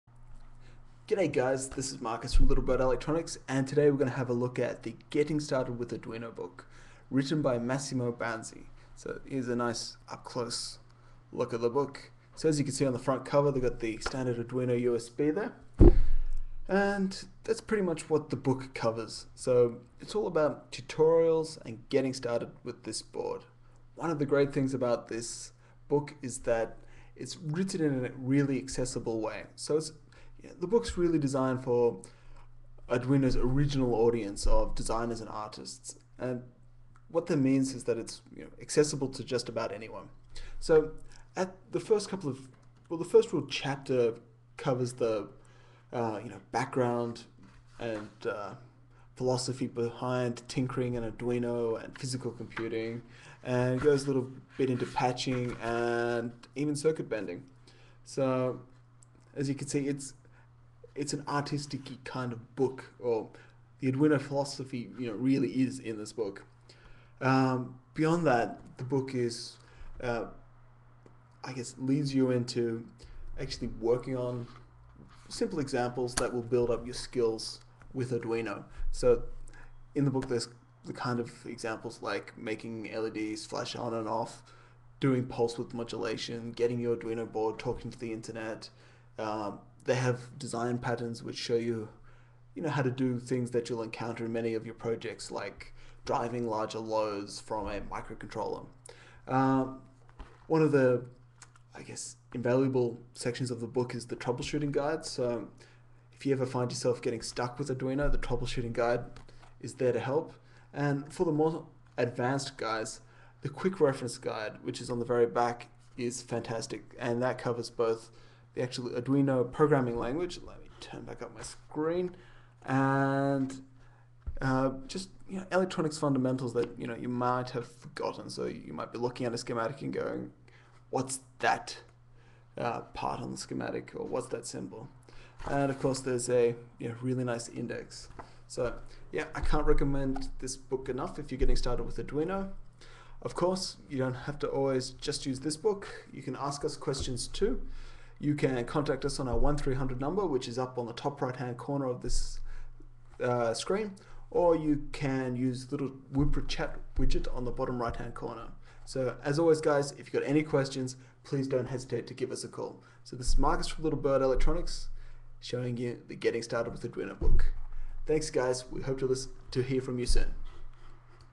Getting Started with Arduino Book BOK-09301
gives us a quick run through of the Getting Started with Arduino Book by Massimo Banzi.